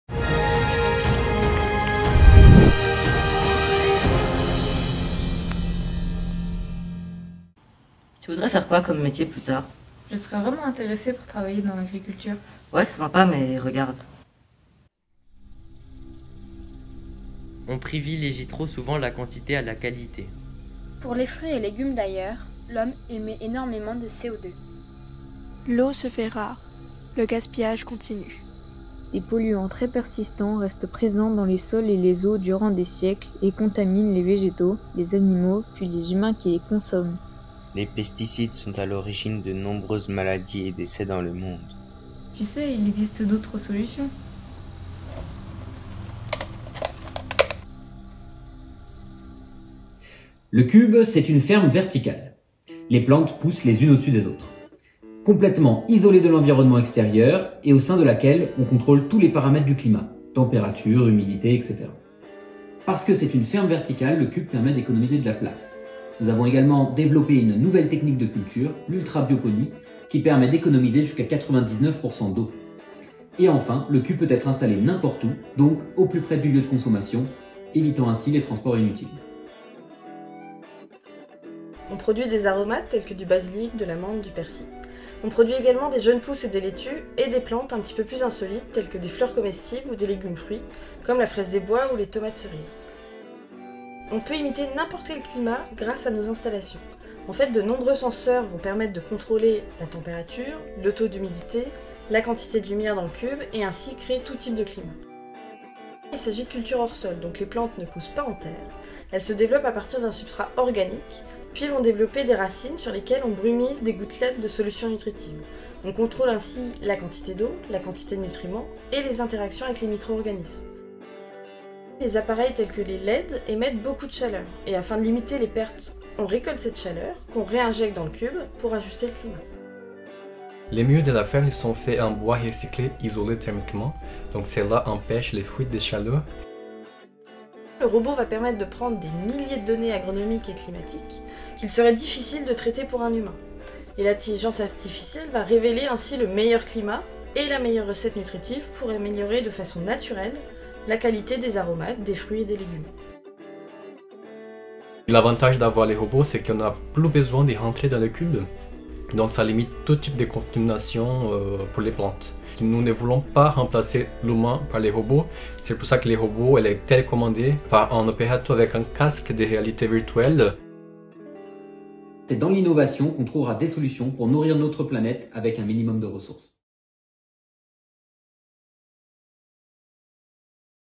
"Agri-future"
Vidéo réalisée par la classe de 3°A du collège Arthur Rimbaud, dans le cadre du Concours "Je filme le métier qui me plaît 2021"